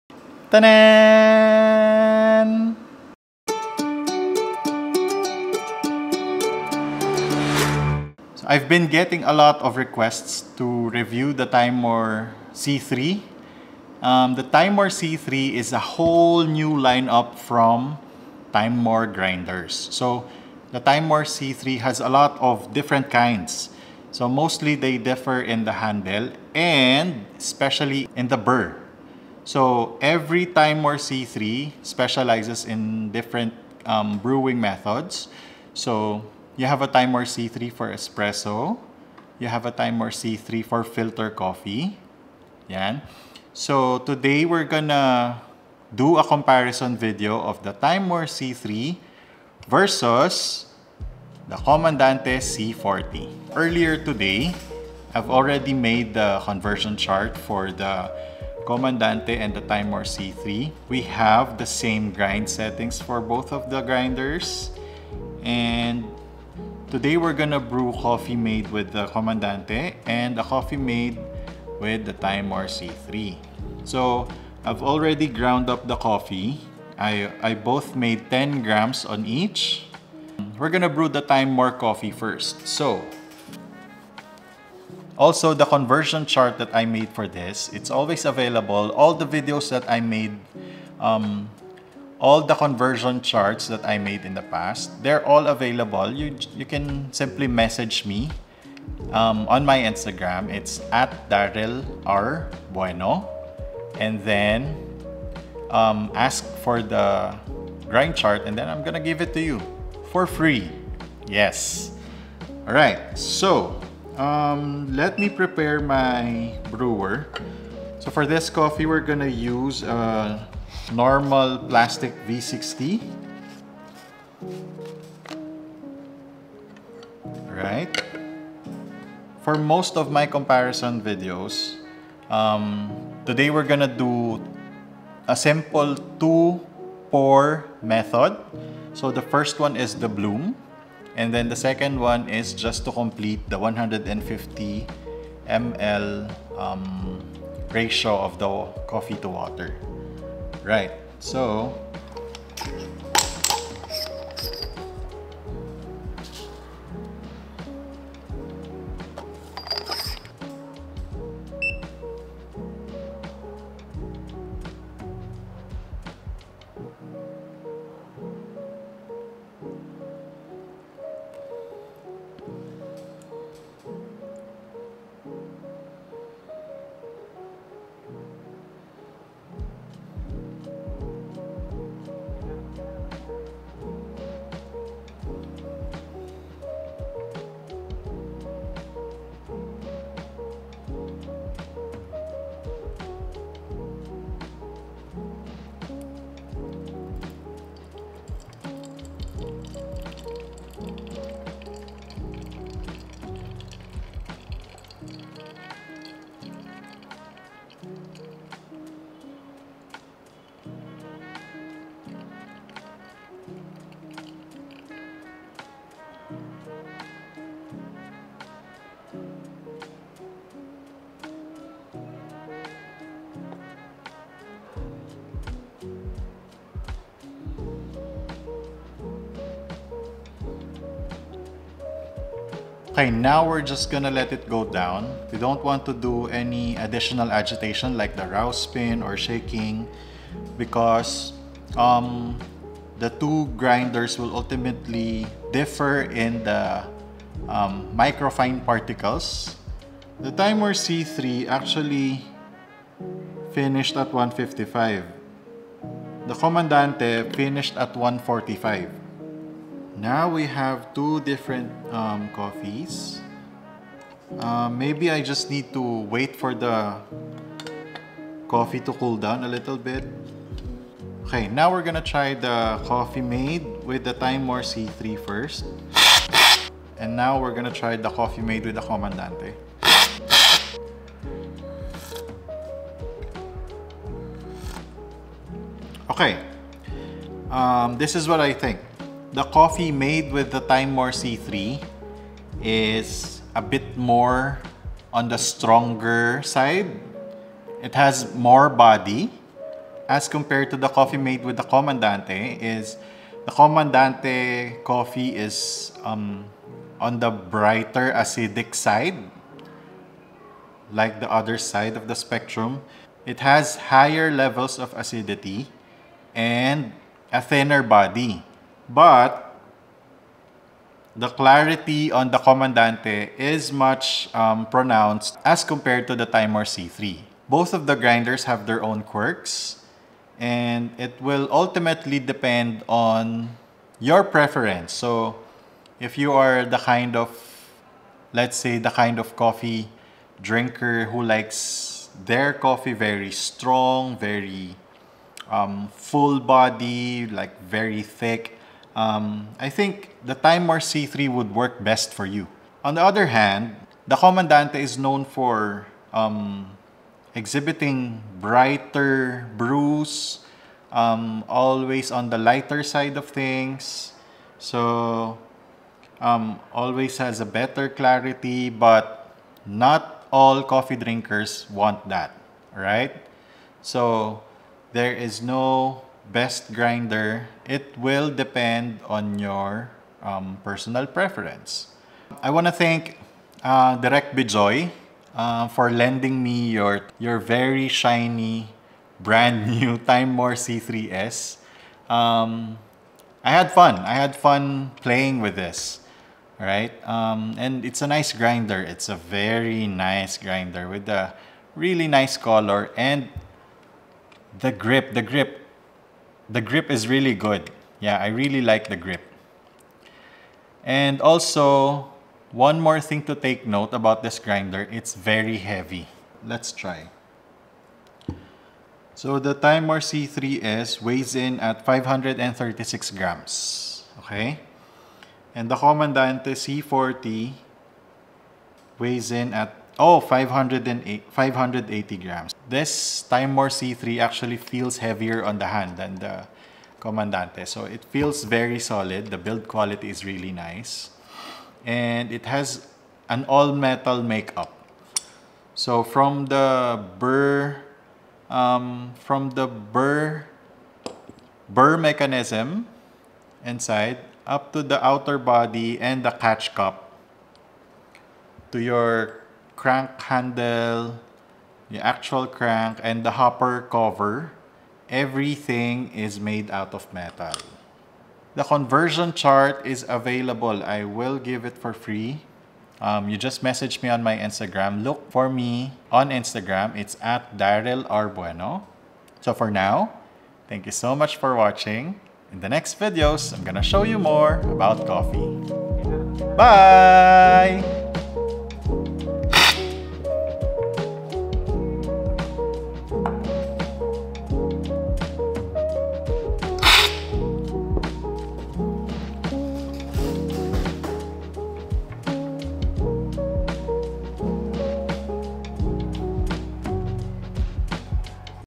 Timemore C3s Hand Grinder
A lot of viewers has been asking for a review of the newly released Timemore C3 - so today we are checking it out and we are also doing a comparison brew of the Timemore C3 vs the Comandante C40.